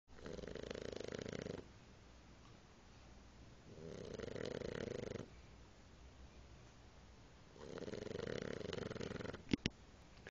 French Bulldog Sleeping With Her Eyes Open
Yes, Skipper the French Bulldog is really asleep in this video!